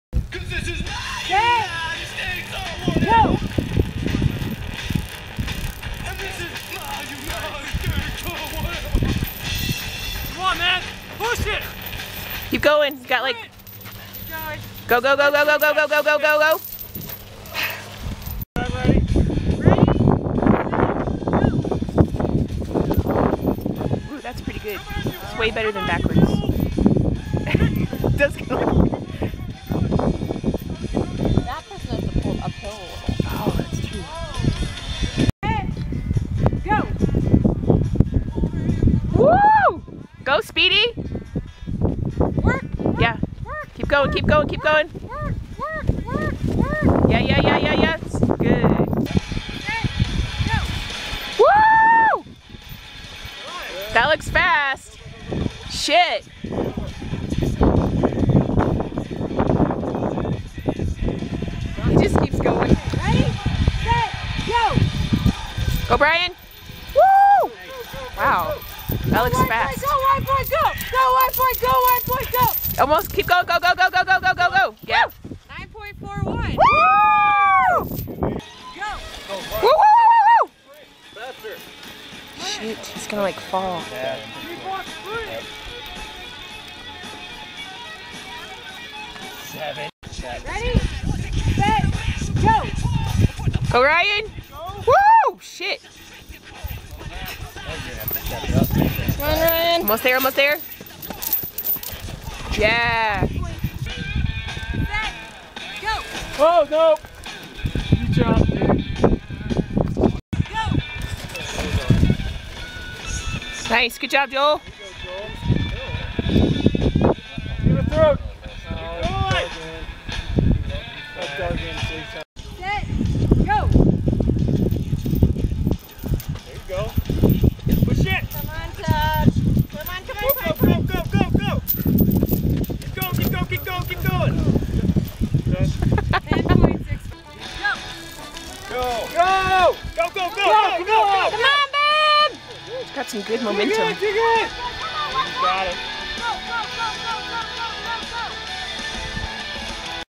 The Tree Pull - Event 2 - 2011 Mountain Man Competition
1st place - 7.94 seconds - Justin
2nd place - 9.06 seconds - Ryan
3rd place - 9.41 seconds - Bryan
4th place - 9.43 seconds - Joel
5th place - 9.81 seconds - Todd